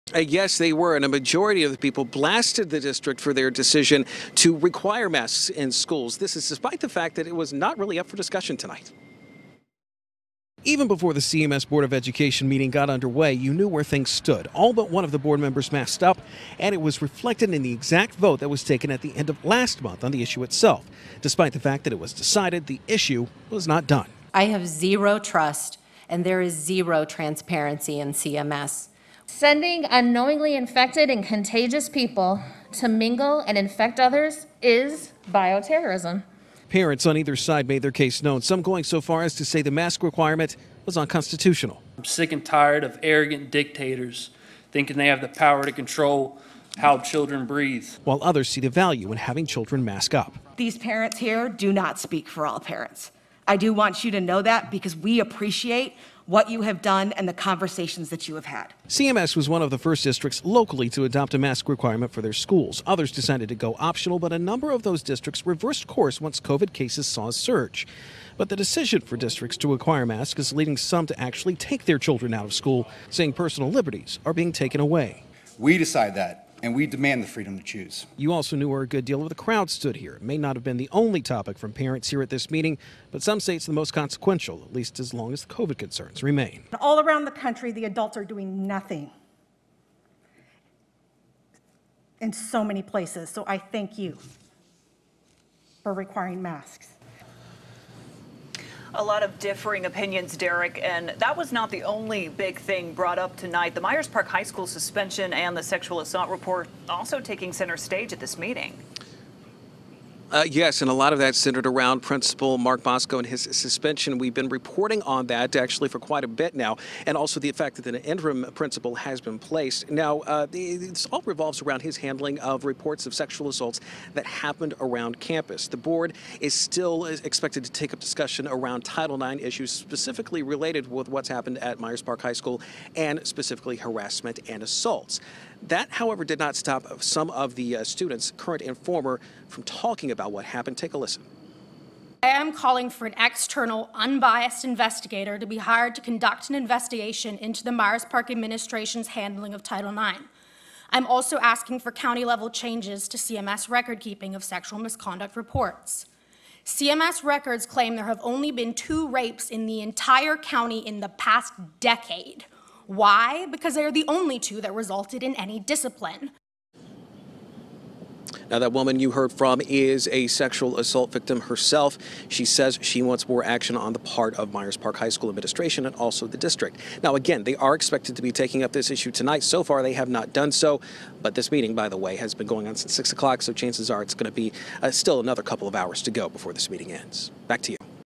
CMS discusses Myers Park HS sex assault reports
CMS discussed Title IX and issues related to harassment, however, students say more needs to be done.